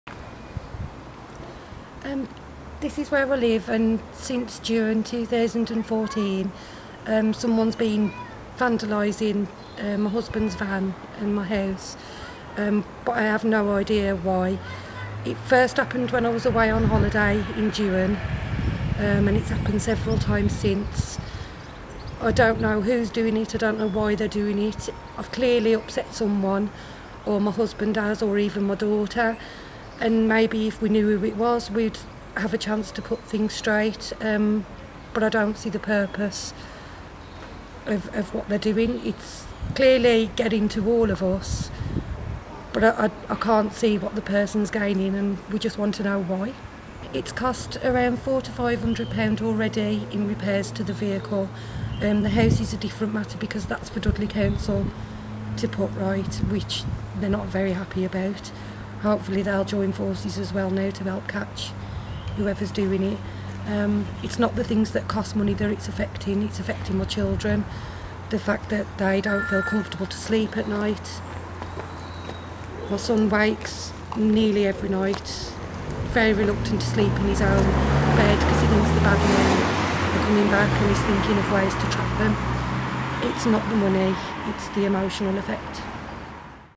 Yob who tormented family for months caught on CCTV daubing outside home in black PAINT
Kelly Mole, 37, installed security cameras in a bid to catch the vandal who has attacked her house and husband’s van five times since last July.

CCTV captured a masked thug spending 20 minutes crudely defacing the walls of her home in Sedgley, West Mids., while an accomplice kept a lookout at 3.50am on February 6.

Kelly, who lives with husband Roger, 48, a builder, and their children Ben, six, and 13-year-old daughter Jade, said: "I have no idea why we are being singled out. We’re being terrorised.

"I have spoken to every single person I know and I am at a total loss as to why it's happened. There is no reason behind it.

"I just wish they had the guts to knock on my door and tell me what I and my family are supposed to have done.